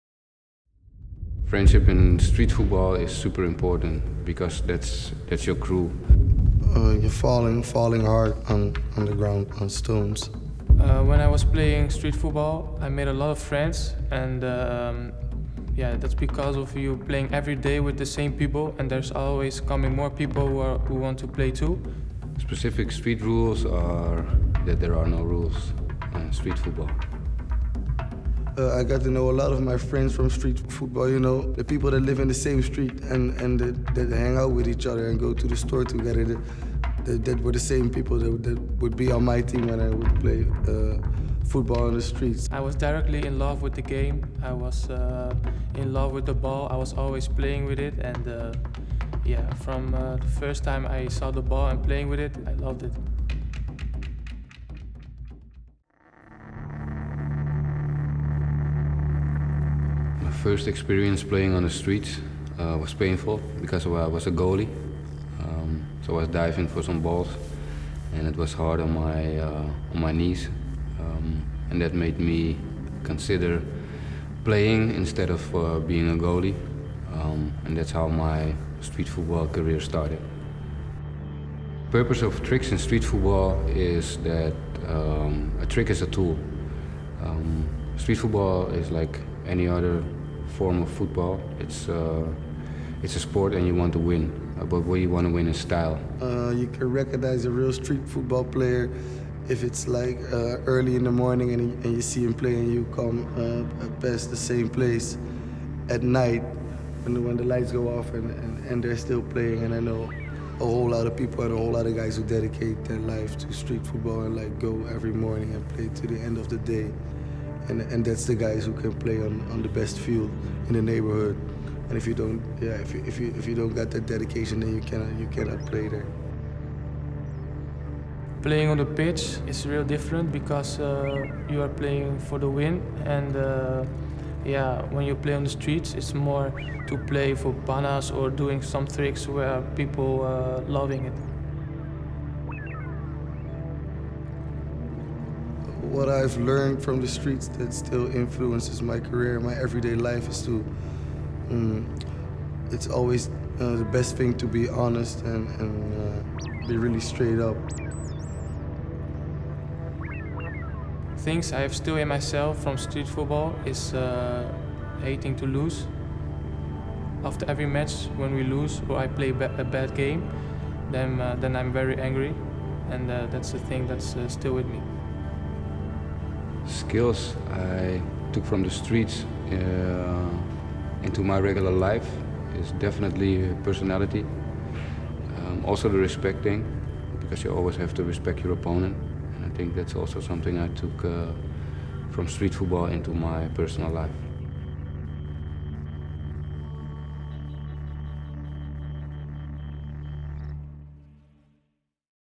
Nike Football X Uncovers The Secrets of Street Football and MyOskarStyle
Ahead of the Amsterdam Nike Football X Winner Stays European Final on December 13, HYPEBEAST spent time with Netherlands striker Anwar El-Ghazi, Street Football legend Edward van Gils and music artist Ronnie Flex for their views on what straatvoetbal means to them.